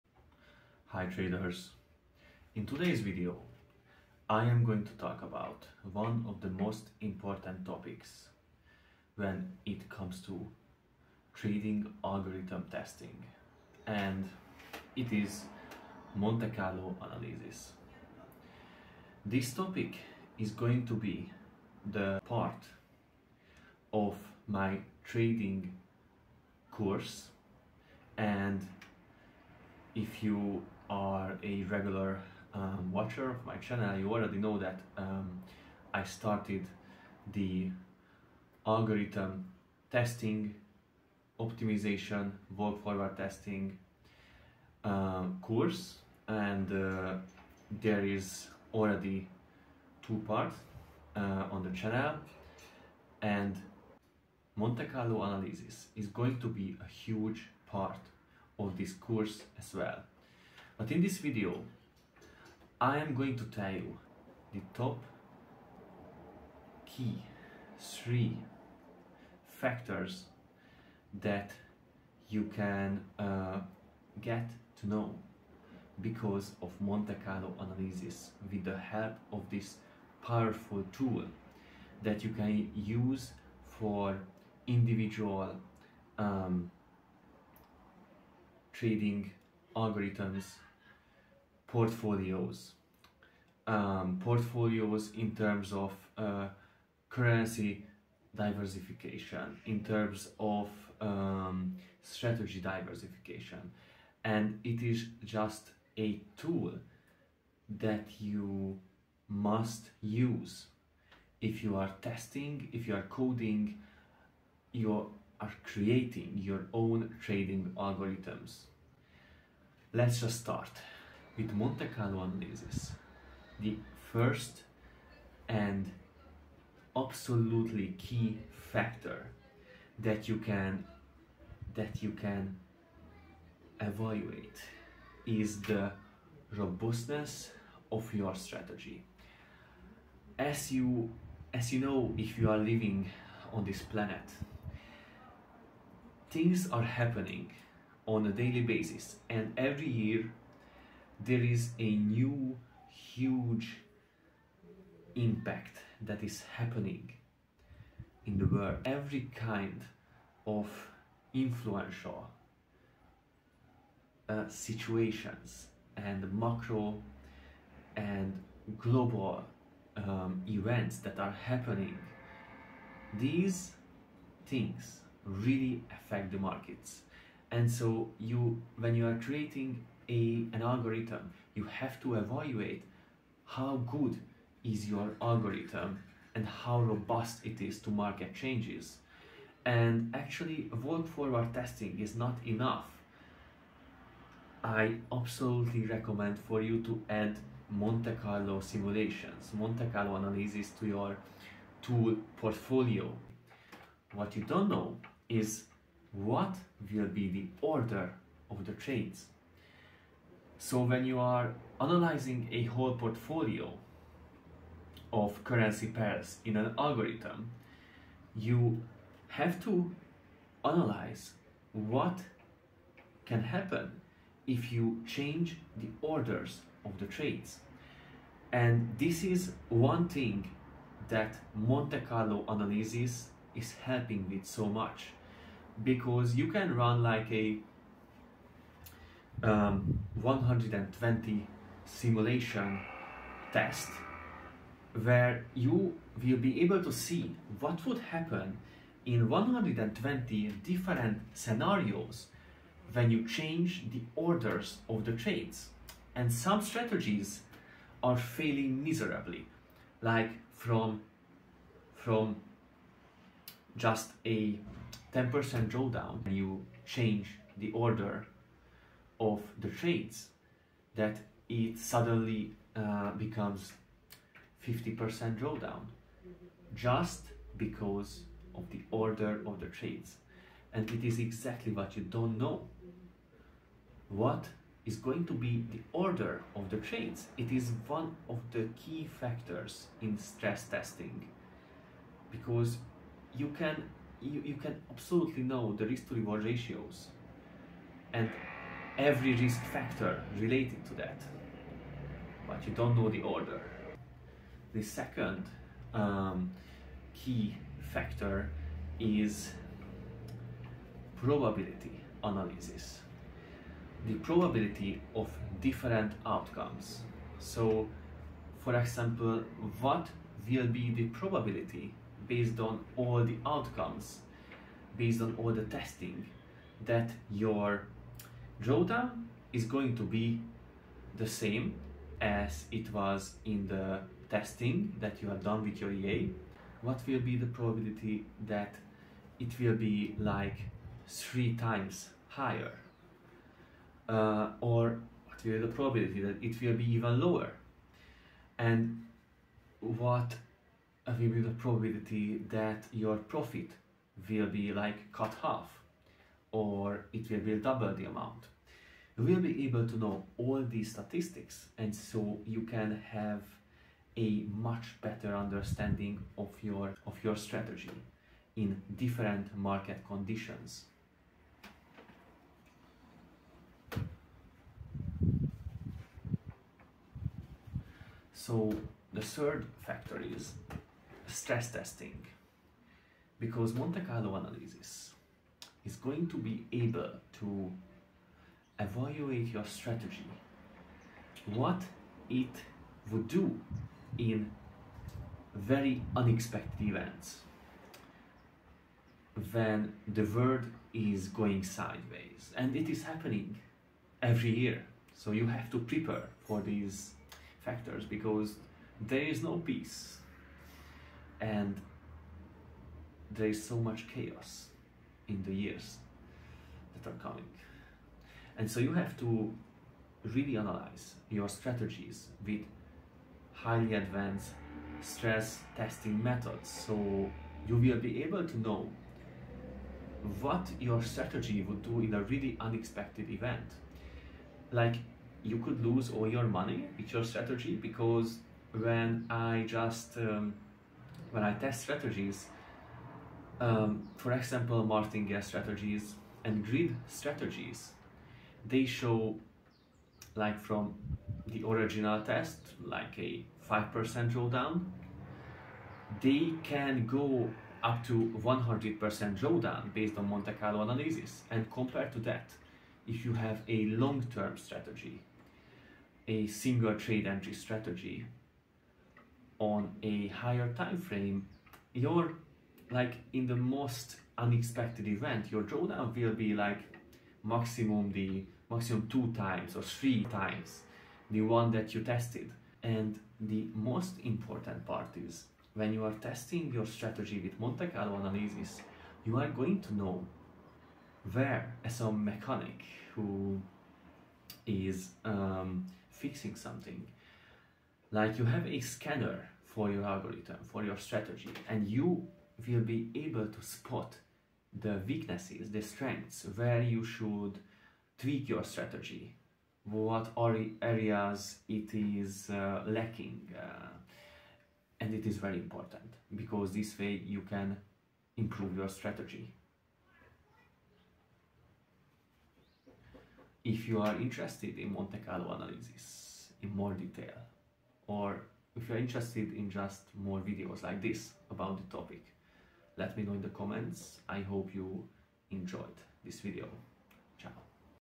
The Monte Carlo Analysis Advantage: Improving Forex Trading EAs
The Monte Carlo Analysis Advantage: Improving Trading Systems

The Nomad Trader EA Forex Trading Robot 99.9% tick data Backtest for 15 years! My Most Complex EA!

New Developement my Best price action EA yet for MT5 IN Store